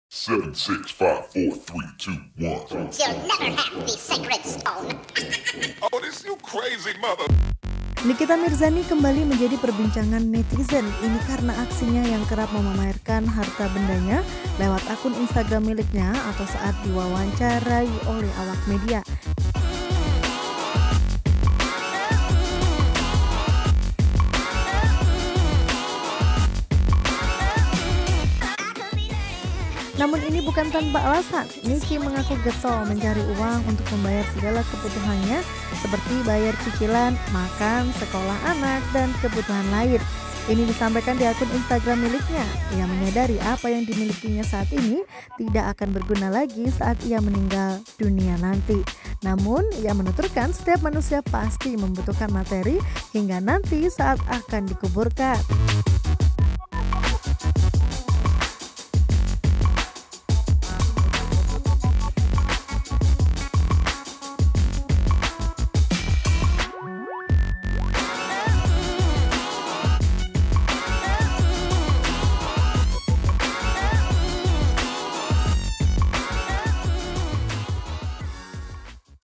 Dibilang Suka Pamer, Nikita Mirzani: Mati Saja Masih Butuh Uang Buat Mengubur
jpnn.com, JAKARTA - Nikita Mirzani kembali jadi pembicaraan warganet lantaran aksinya yang kerap pamer harta bendanya lewat akun Instagram miliknya maupun saat diwawancarai awak media...